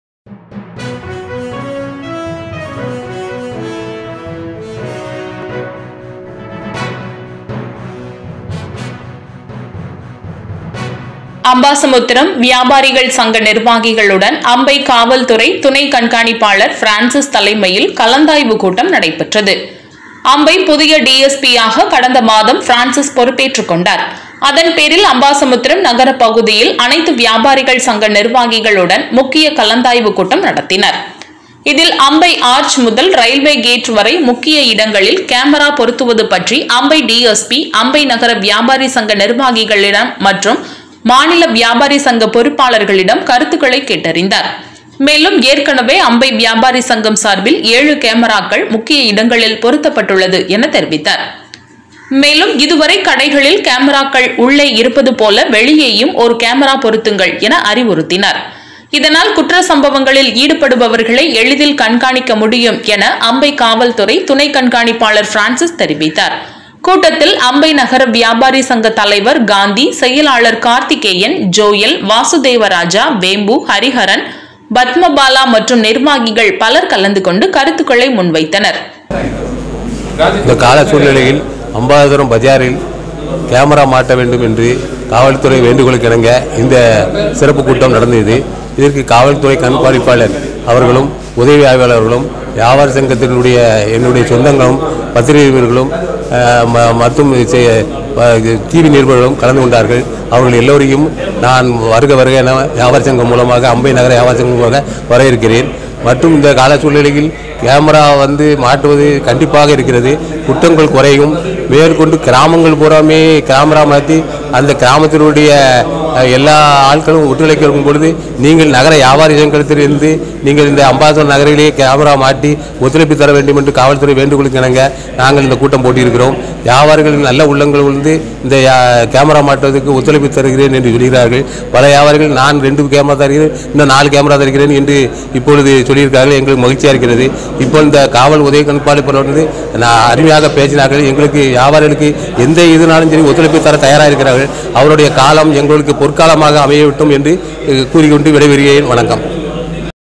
AMBASASAMUDRAM | அம்பை |DSP | TN POLICE | டிஎஸ்பி வியாபாரி சங்க நிர்வாகிகளுடன் கலந்தாய்வு
நெல்லை மாவட்டம் செப்:21 அம்பாசமுத்திரம்
வியாபாரிகள் சங்க நிர்வாகிகளுடன்
அம்பை காவல்துறை துணைக் கண்காணிப்பாளர் பிரான்ஸிஸ்  தலைமையில் கலந்தாய்வு கூட்டம் நடைபெற்றது.

 அம்பை புதிய டி. எஸ் .பி .யாக கடந்த மாதம் பிரான்ஸிஸ் பொறுப்பேற்றுக் கொண்டர்.
அதன் பேரில் அம்பாசமுத்திரம் நகர பகுதியில் அனைத்து வியாபாரிகள் சங்க நிர்வாகிகள் உடன் முக்கிய கலந்தாய்வுக் கூட்டம் நடத்தினர்.

இதில் அம்பை ஆர்ச் முதல் ரயில்வே கேட் வரை முக்கிய இடங்களில் கேமிரா பொருத்துவது பற்றி அம்பை டிஎஸ்பி. அம்பை நகர வியாபாரி சங்க நிர்வாகிகளிடம் மற்றும் மாநில வியாபாரி சங்க பொறுப்பாளர் களிடம் கருத்துக்களை கேட்டறிந்தார்.

மேலும் எற்கனவே  அம்பை வியாபாரி சங்கம் சார்பில் ஏழு கேமிராக்கள் முக்கிய இடங்களில் பொருத்தப்பட்டுள்ளது  என தெரிவித்தனர்..

மேலும்.
 இது வரை கடைகளில்  கேமிராக்கள் உள்ளே இருப்பது போல வெளியேயும் ஓர் கேமிரா  பொருத்துங்கள் என அறிவுருத்தினார்.

இதனால் குற்ற சம்பவங்களில் ஈடுபடுவர்களை எளிதில் கண்காணிக்க முடியும் என அம்பை காவல்துறை துணை கண்காணிப்பாளர் பிரான்ஸிஸ்  தெரிவித்தார்.

கூட்டத்தில் அம்பை நகர வியாபாரி  சங்க தலைவர் காந்தி, செயலாளர் கார்த்திகேயன் , ஜோயல், வாசுதேவ ராஜா.வேம்பு.ஹரிஹரன்.
  பத்ம பாலா மற்றும் நிர்வாகிகள் பலர் கலந்து கொண்டு  கருத்துக்களை முன் வைத்தனர்..